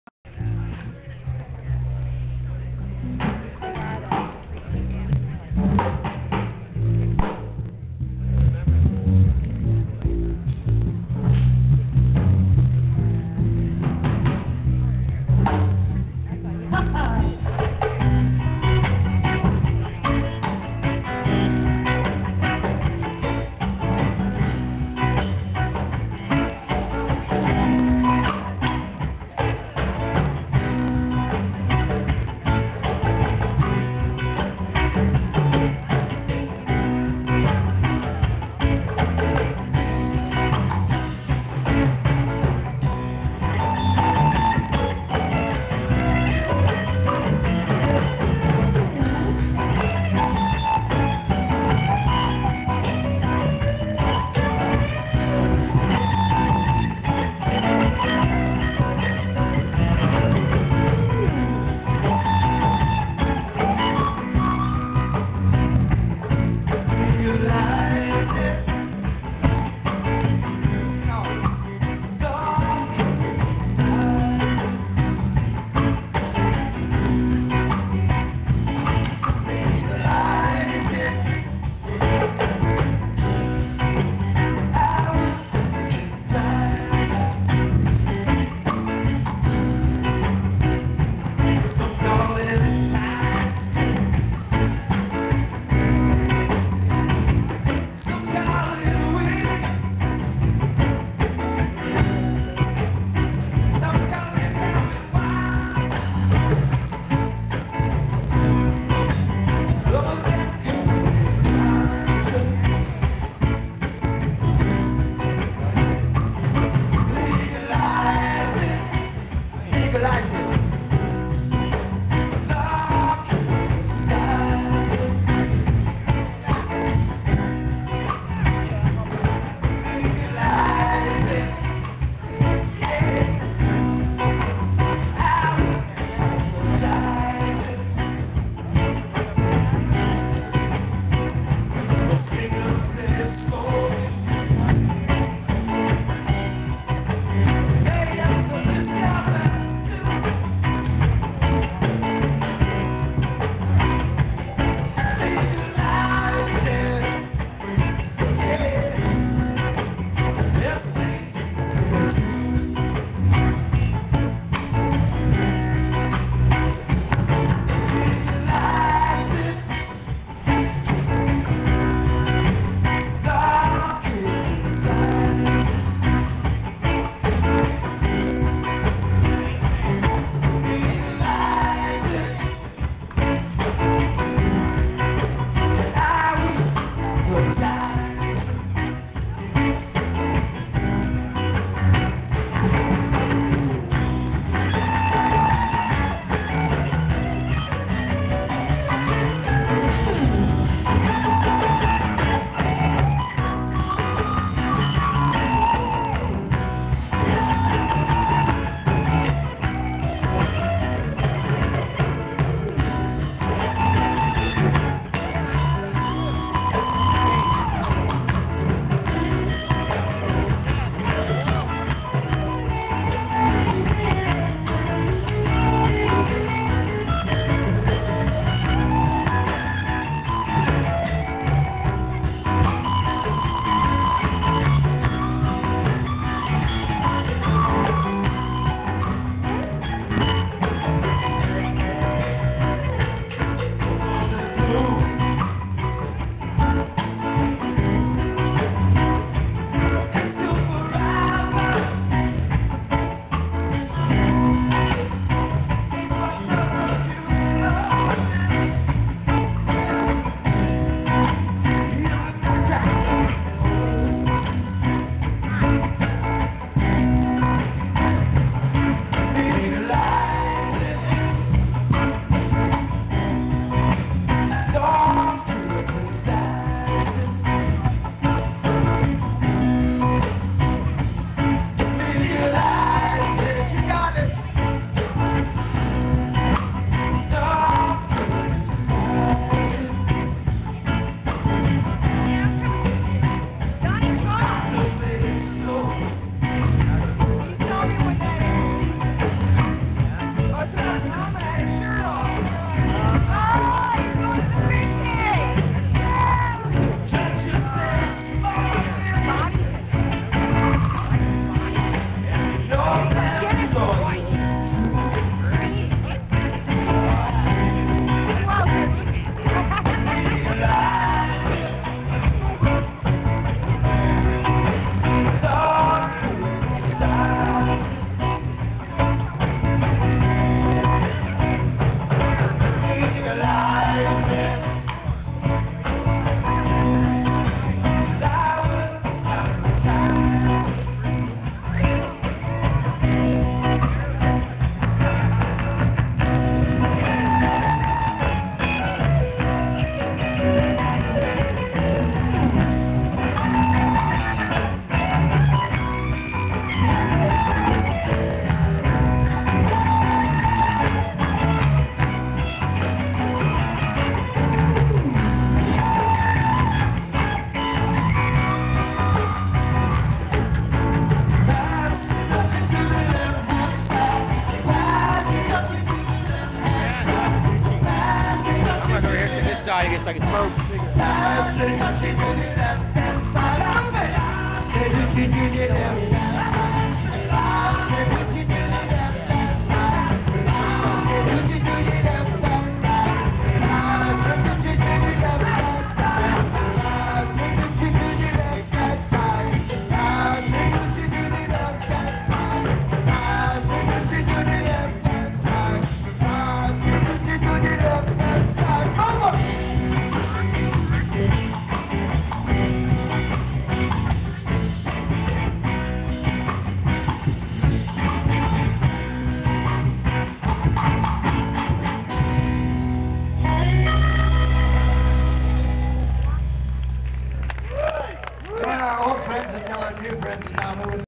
Willie Jack & The Northern Lights - A Bear's Picnic 4 - 8/14/09
My apologies for the crackiling noise in the audio. And for not gettin more video of these guys! Sorry! Great band!!